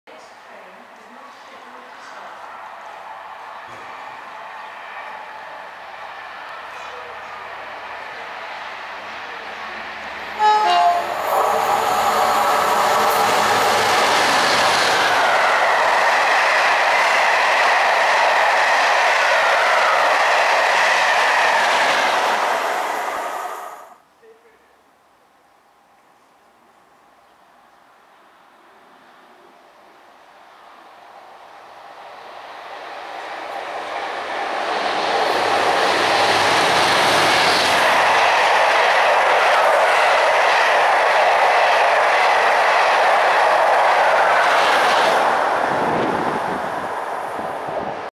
57314 top and tail with 57315 passes Bridgwater with the Exmoor Coast Statesman on 19th July 2014
57314 top and tail with 57315 seen passing Bridgwater with 1Z57 'Exmoor Coast Statesman' from Sheffield to Minehead.

On the return 57315 top and tail with 57314 seen passing Bridgwater with 1Z58 'Exmoor Coast Statesman' from Minehead to Chesterfield.